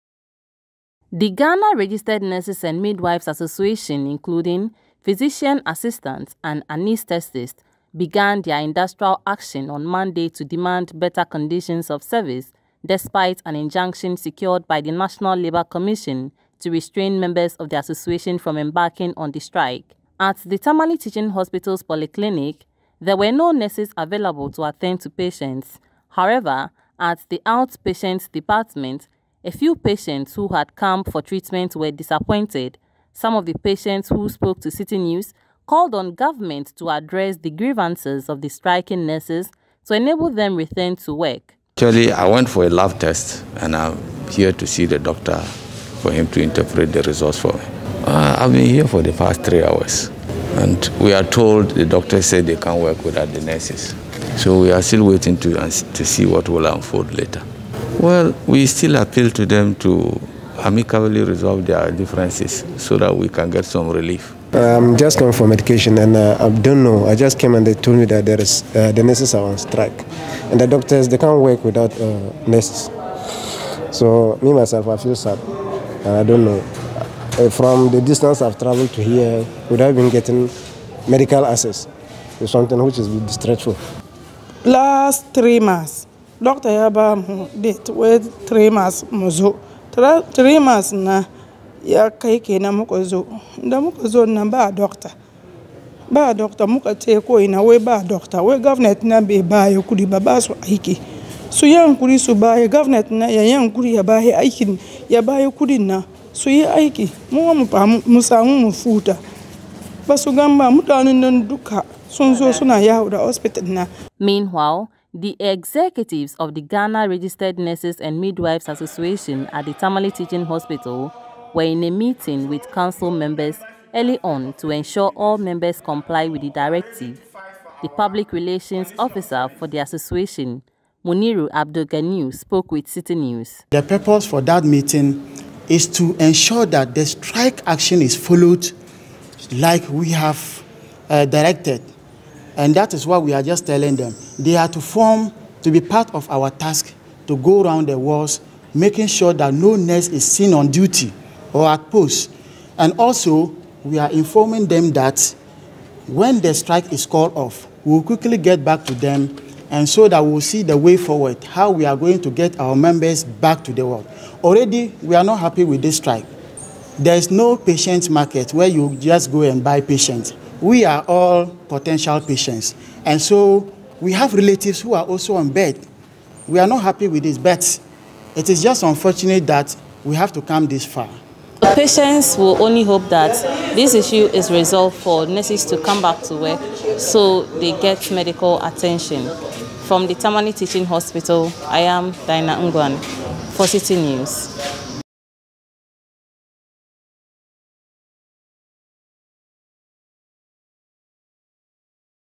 Patients stranded at Tamale teaching hospital as nurses abandon post | Citi Newsroom
The Ghana Registered Nurses and Midwives Association including Physicians Assistants and Anesthetists begun their industrial action on Monday  to demand better conditions of service despite an injunction secured by the National Labour Commission to restrain members of the association from embarking on the strike.

At the Tamale Teaching Hospital's Poly Clinic, there were no nurses available to attend to patients.